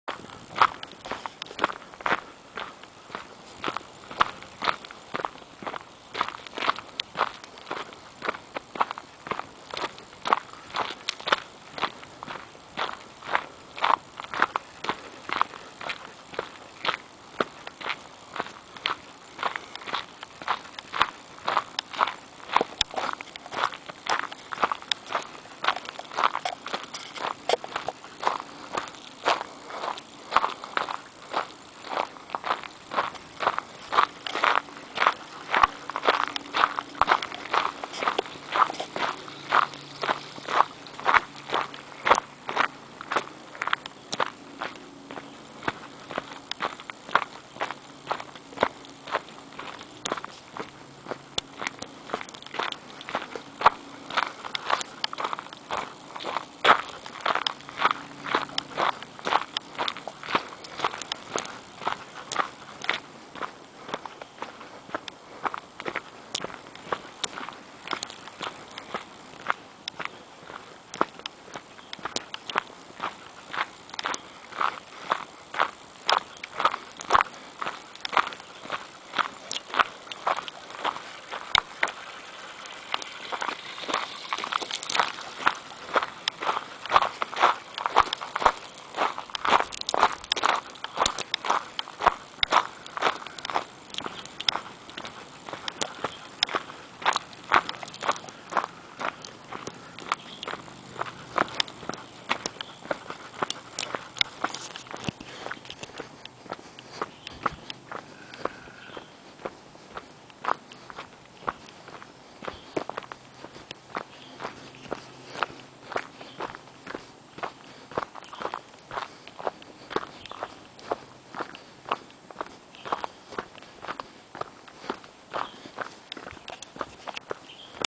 Tiger Mountain Trail
Walking down the trail on Tiger Mountain.  This one is West Tiger Trail.  Nothing special happens here.